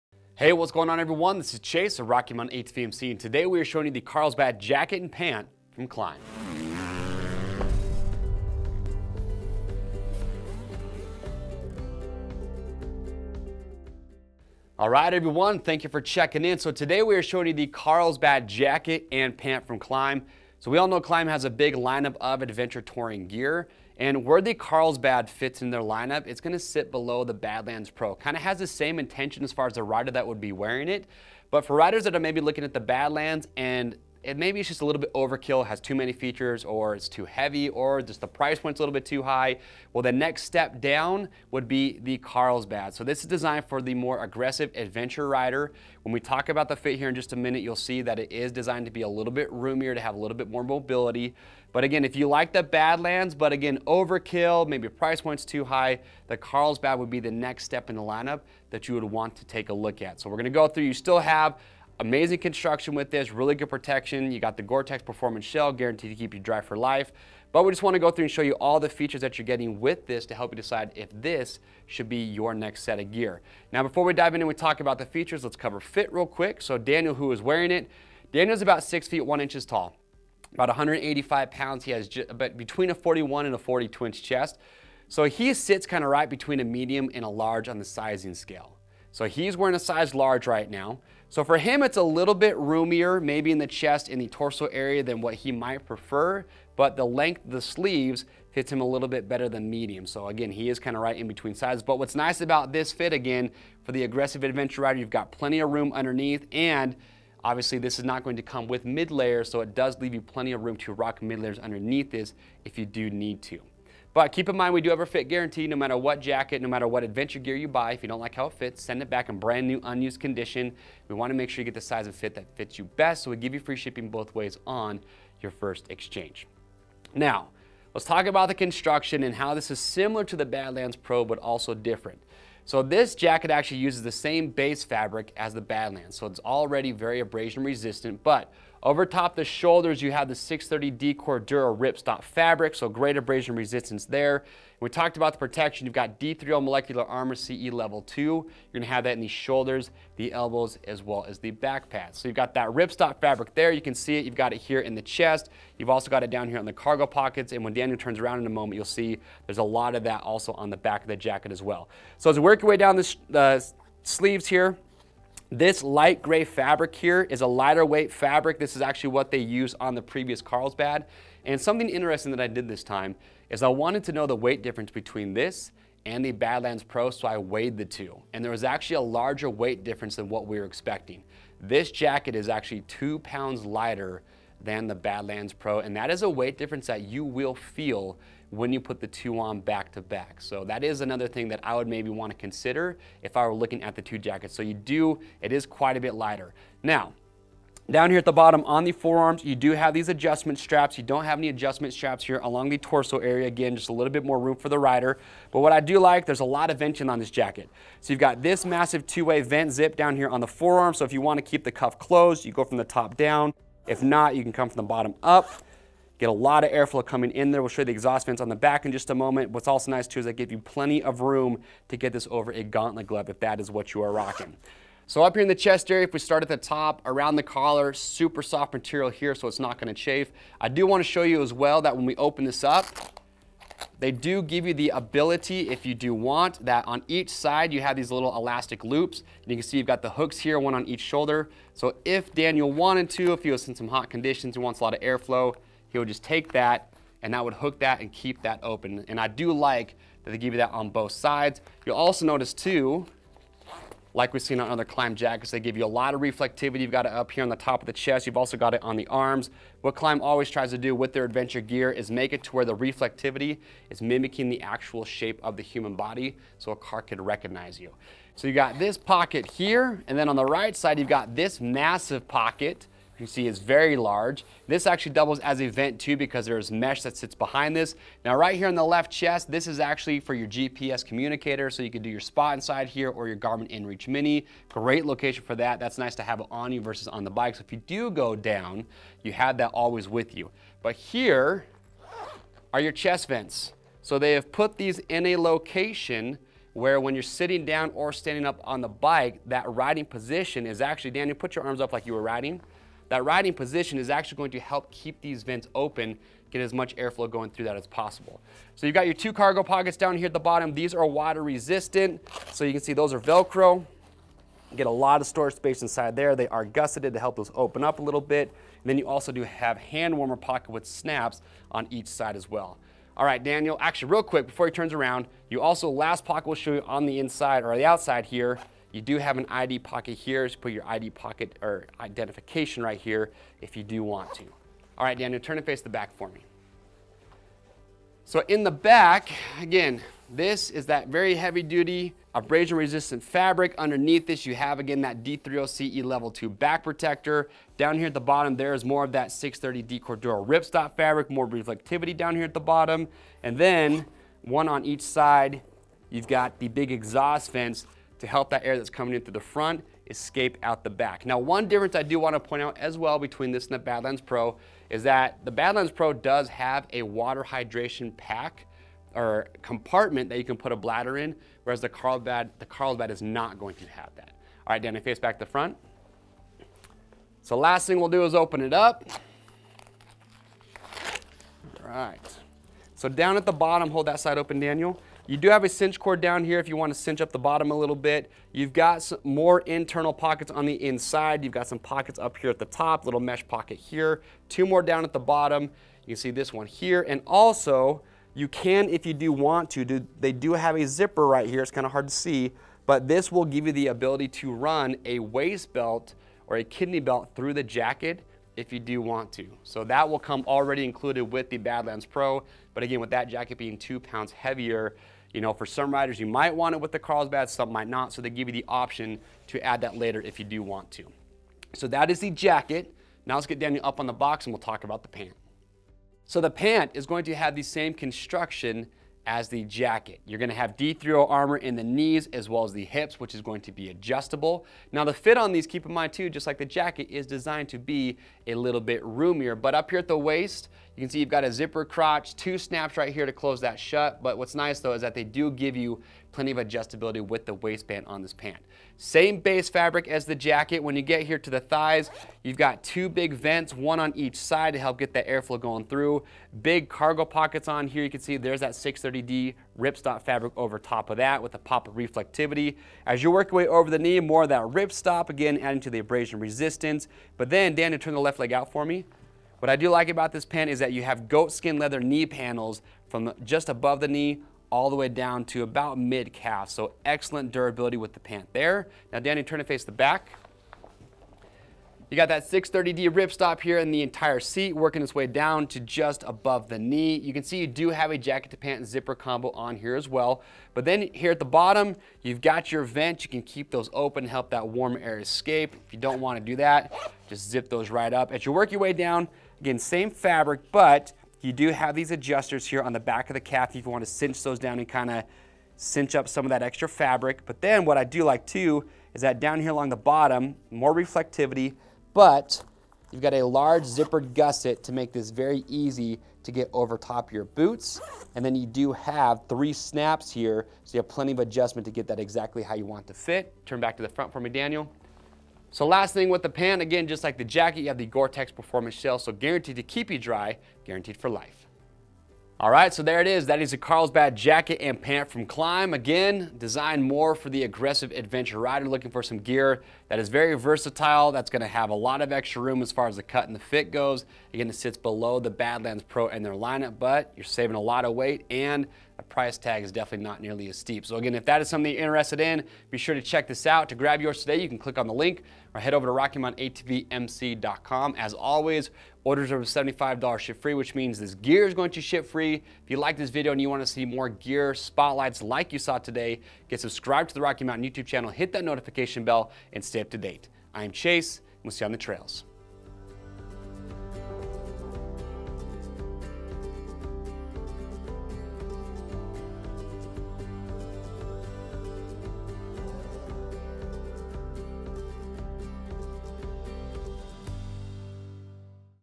Klim Carlsbad ADV Motorcycle Jacket & Pant
The Carlsbad Jacket and Pant have been redesigned and built to an even higher standard with improved ventilation, greater abrasion resistance, more refined storage options and improved impact protection. The Klim Carlsbad gear was built for the hard-charging adventure rider. Take a closer look as Chase gives a product spotlight and goes over all the new features built into the Klim Carlsbad ADV Motorcycle Jacket and Pant.